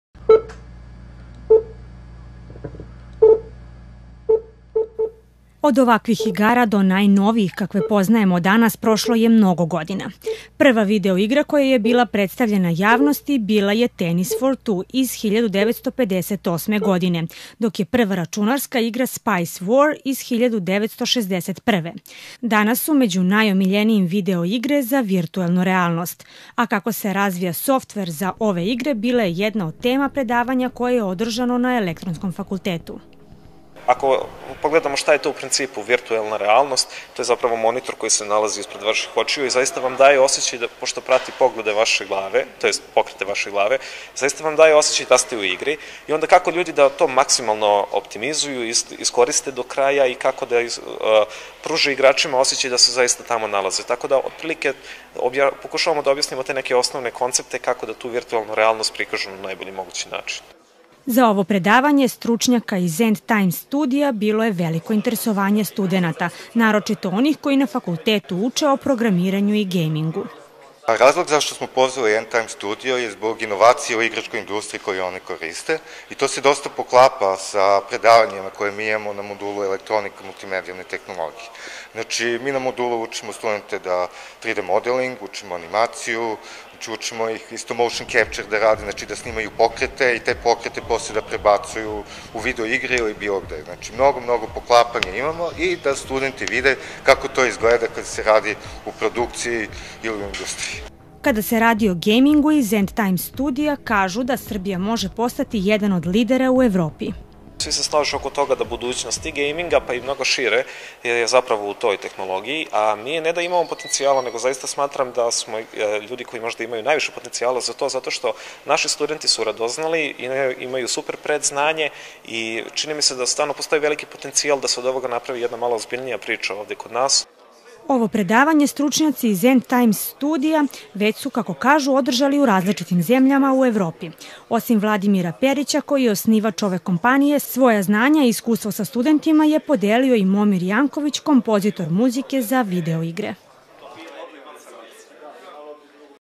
Endtimes studio Multimedijalne tehnologije, prilog NTV
Razlog zbog kojeg smo pozvali Endtimes studio da održe predavanje "Razvoj video igara za virtuelnu realnost" su inovacije u igračkoj industriji i to se dosta se poklapa sa našim planom i programom modula Elektronika/ Multimedijalne tehnologije. Prilog NTV-a.
Predavači:
Vladimir Perić, osnivač, direktor i lead progammer
Miloš Novak, sound designer